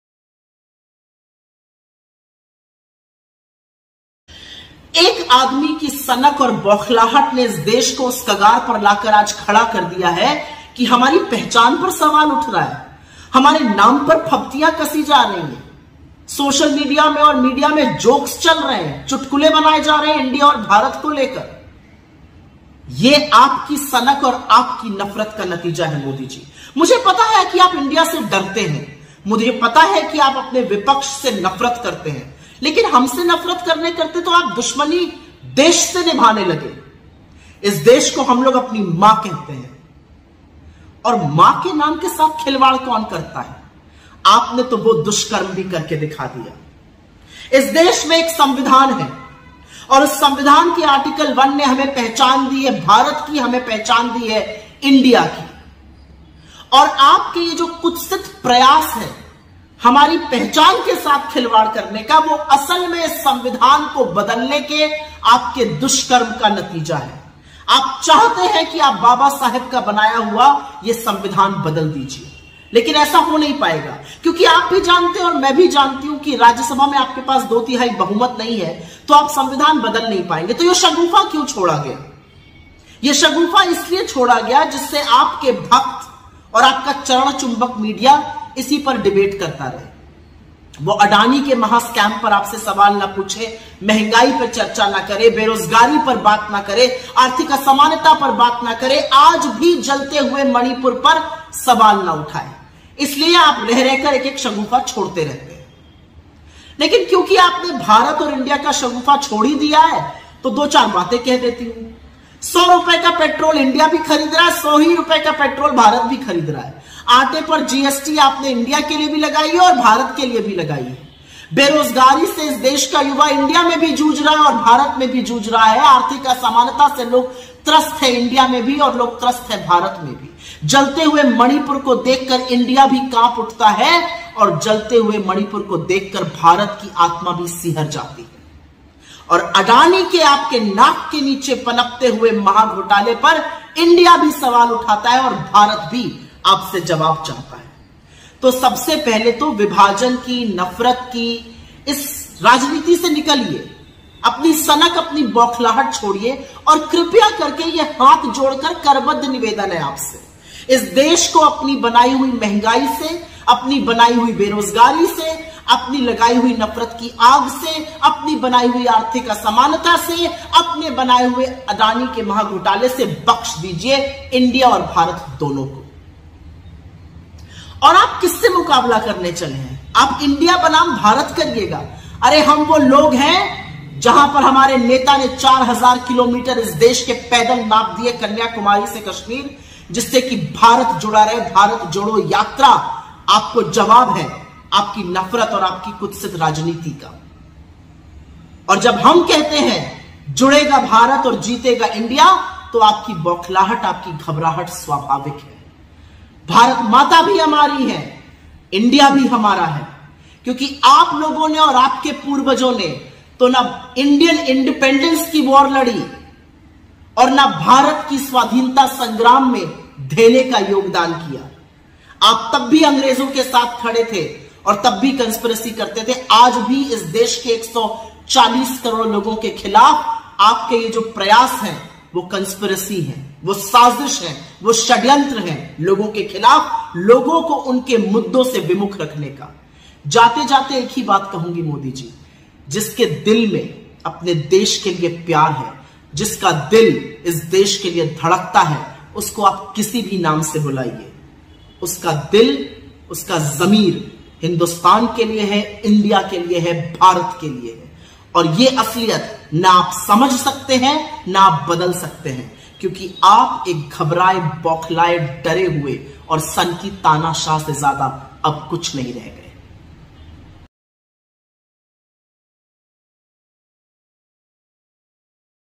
INDIA vs BHARAT: बाबा साहब के संविधान को बदलना चाहती है मोदी सरकार: कांग्रेस
INDIA to be removed from Constitution?
देश से दुश्मनी निभाने लगे हैं पीएम मोदी
INDIA नाम को संविधान से हटाया जाएगा?
Preparation to remove INDIA from the Constitution! Modi government wants to change Baba Saheb's Constitution: Congress